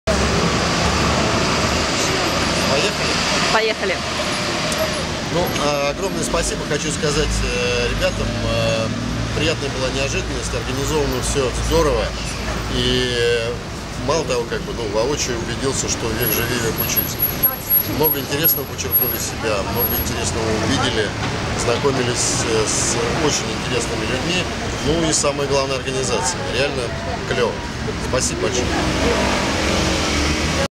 Образовательные программы Food & Success
Инновационный викенд в Барселоне для рестораторов и поваров. Отзыв ресторатора Андрея Елисеева.